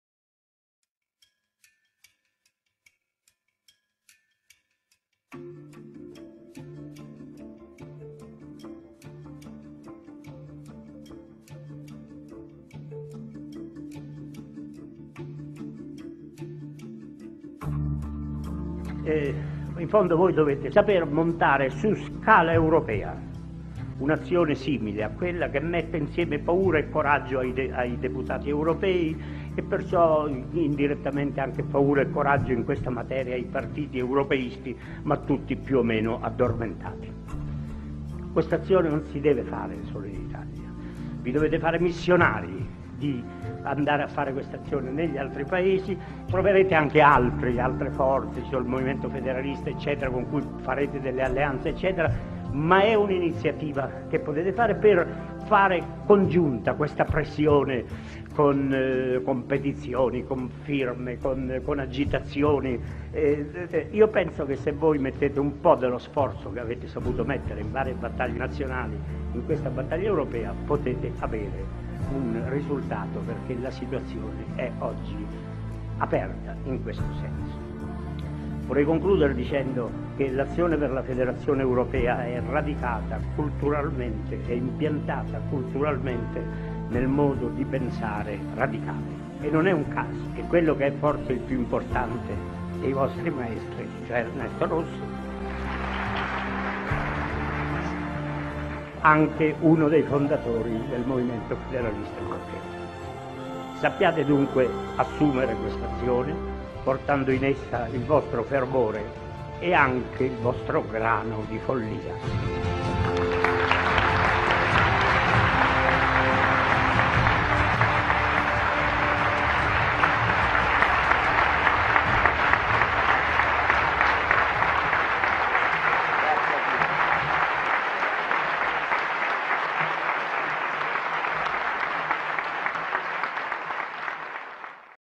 Altiero Spinelli's Testament
Speech by Altiero Spinelli at the Congress of the Radical Party (1985), one year before his death